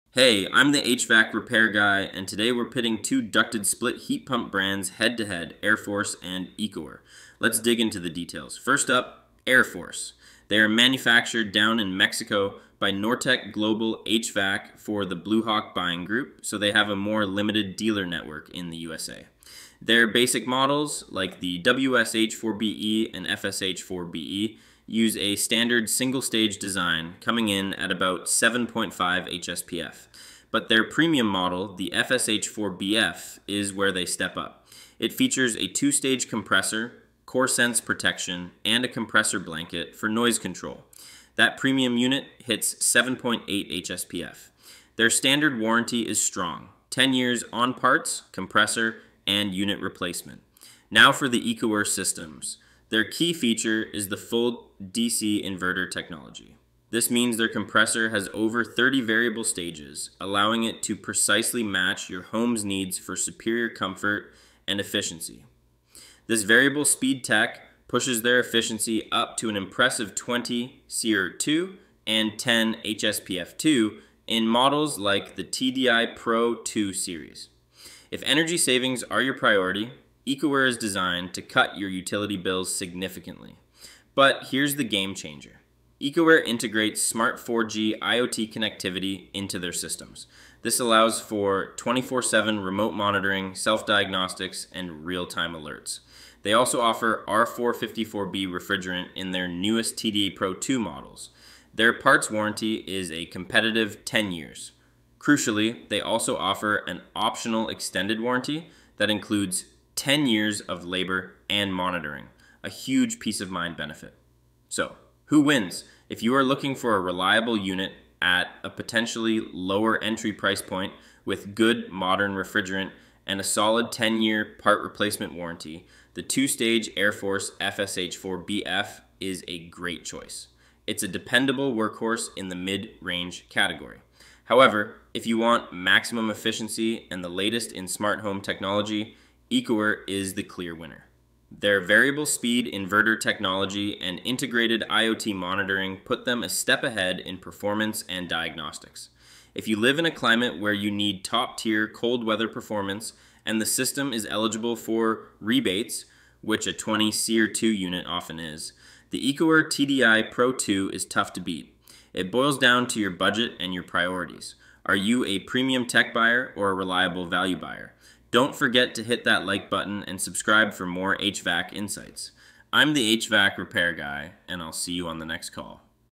AireForce vs. Ecoer Heat Pump Review: Which Ducted Split HVAC is Best for Your Home?
Hey there, I'm the HVAC Repair Guy, and in this video, we're doing a head-to-head heat pump comparison between two major players in the ducted split market: AireForce and Ecoer.

Choosing a new central air system is tough, but I’ll break down the key differences to help you decide which brand is the best fit for your home comfort and budget.

What We Cover:

AireForce Heat Pump: Reviewing their Basic and Premium models (like the FSH4BF), focusing on the two-stage compressor technology, Coresense protection, and their move to the new R454B refrigerant.

Ecoer Heat Pump: Diving deep into their cutting-edge Inverter heat pump technology. Learn why their variable-speed design achieves up to 20 SEER2 and 10 HSPF2 efficiency.

Technology Breakdown: We compare AireForce's two-stage system against Ecoer's Full DC Inverter with IoT connectivity (Smart 4G monitoring) for diagnostics and service.

Warranty & Value: A look at the 10-year parts warranties and Ecoer's optional extended labor/monitoring service.

Final Verdict: We’ll tell you if the reliable mid-range performance of AireForce is enough, or if the high-efficiency, smart features of Ecoer are worth the investment.